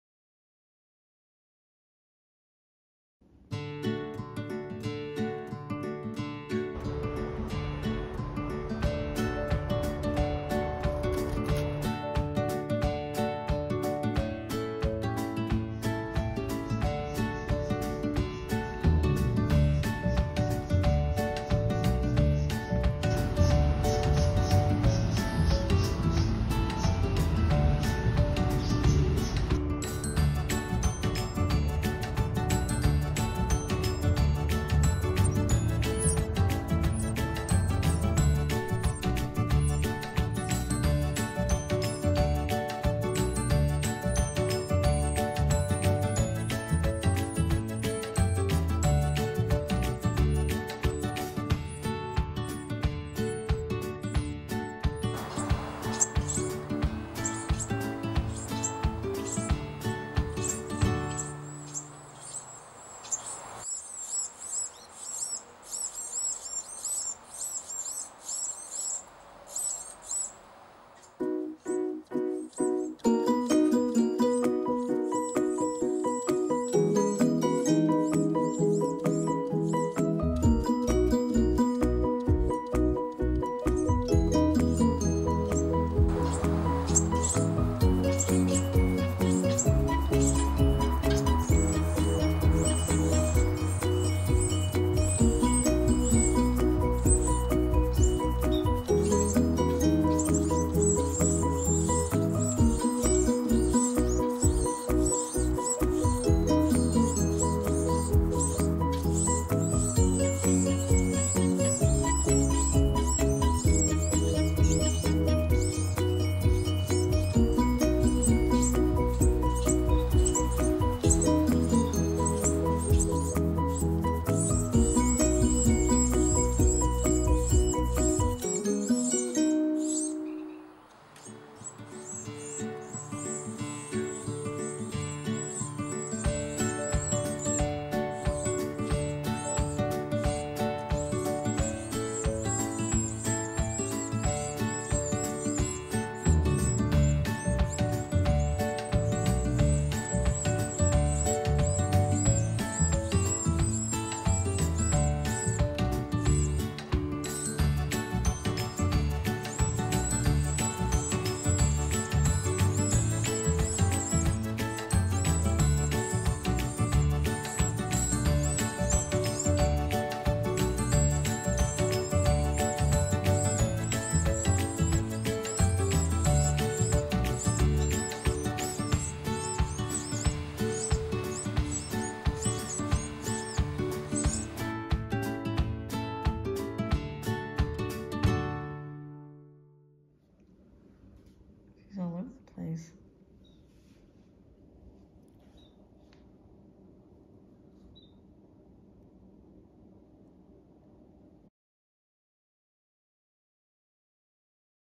Baby Carolina Wrens 2021 SD 480p
A pair of Carolina Wrens built a nest on our back porch, and we got to watch the babies hatch, grow, and feed every day. This is a video of some of our nest watches. Sadly, we missed the fledging. It's so hard to capture that part! But we did see them flying around the yard a couple of weeks later, and it appears all five wrens survived.